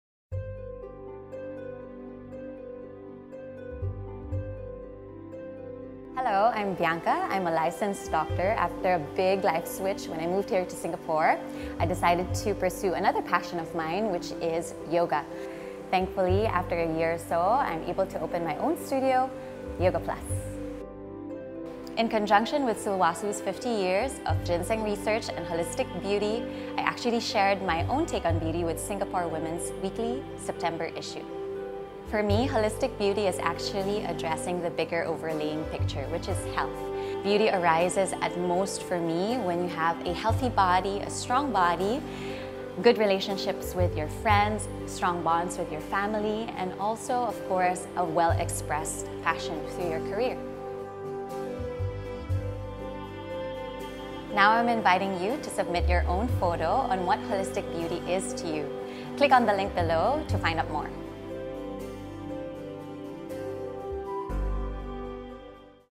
Sulwhasoo Singapore - ‎50 Year Ginseng Research‬ x Bianca Pereche-Gamboa
Sulwhasoo Singapore invited fans to celebrate 50 years of Ginseng Research. To commemorate the ginseng heritage of their brand, popular public figures were invited to share their take on what Holistic Beauty means to them and to encourage fans to participate in a photo contest.

Connect with ATC.social!

Are you an influencer?